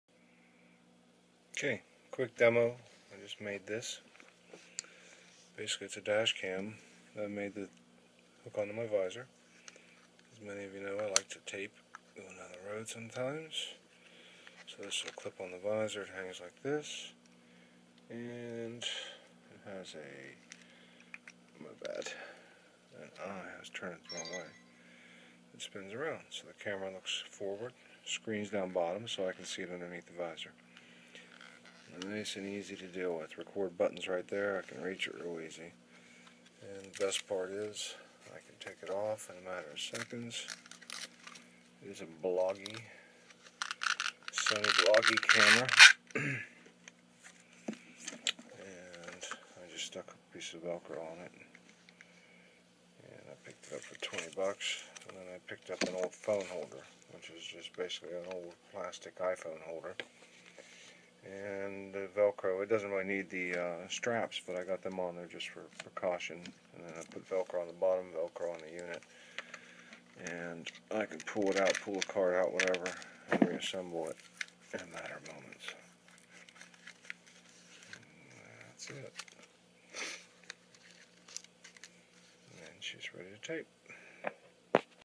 Homemade Bloggie Dashcam
20 for Bloggie and leftover Iphone hard belt clip.